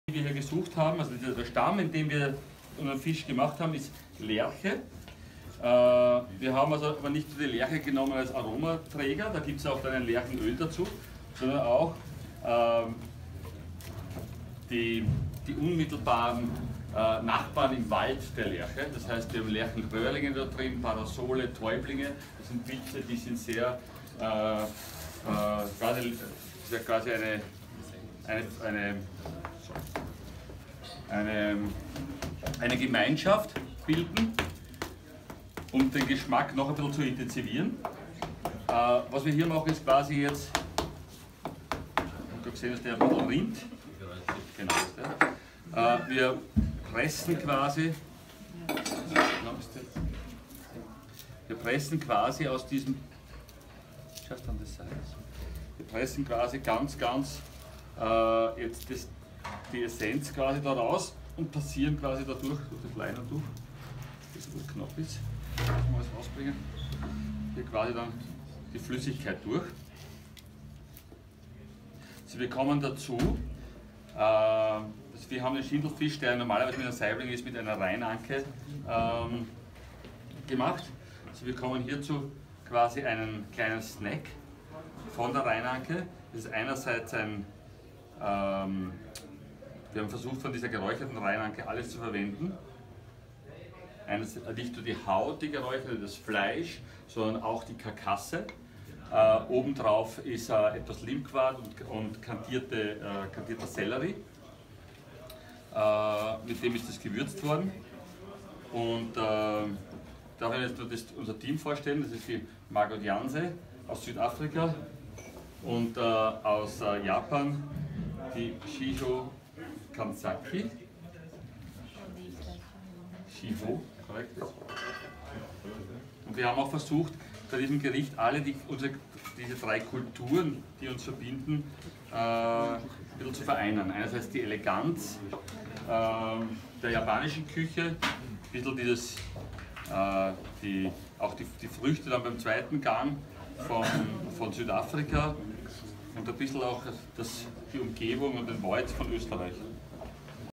Chef Heinz Reitbauer (Steiereck) with Margot Janse and Chiho Kanzaki
Heinz Reitbauer has together with Margot Janse and Chiho Kanzaki created at the Gelinaz goes Upper Austria event a very special dish which he is explaining here.